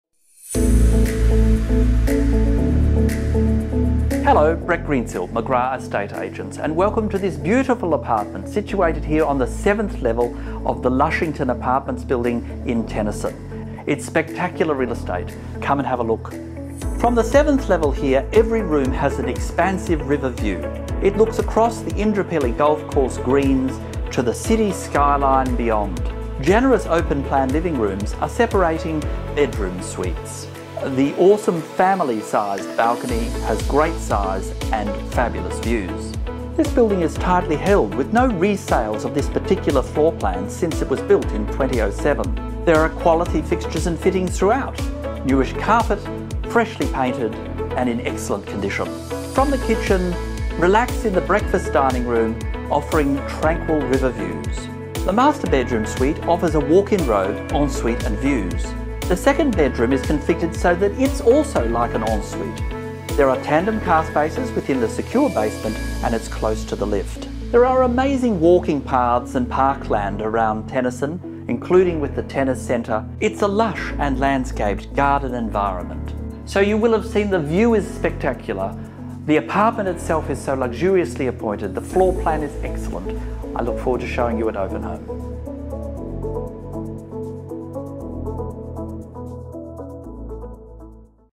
219/205 King Arthur Terrace, Tennyson
This luxurious seventh level riverfront apartment in Mirvac's Lushington building at Tennyson is a prime example of exceptional real estate.  Every room has expansive river views, over the Indooroopilly Golf Course greens to the Brisbane City skyline beyond.